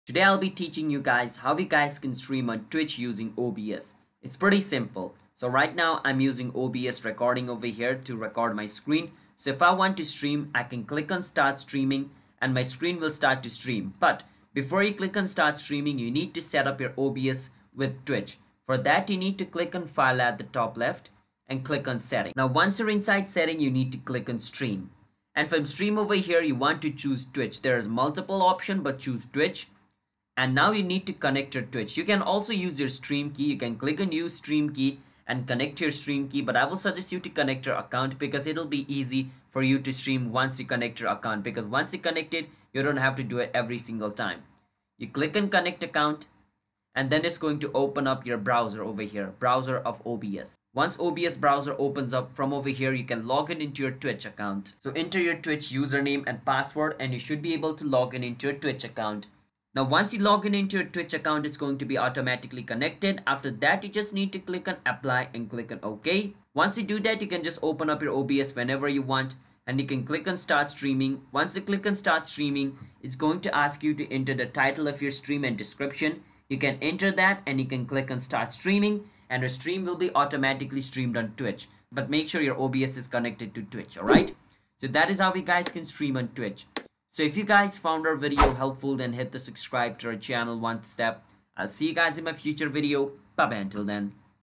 How to Stream on Twitch (STEP-BY-STEP)
This video walks you through the step by step process of how tostream on twitch. Learn how to start your own Twitch streaming journey with this step-by-step tutorial! In this video, we'll cover everything from setting up your stream to engaging with your audience. Whether you're a beginner or looking to up your streaming game, this guide has you covered.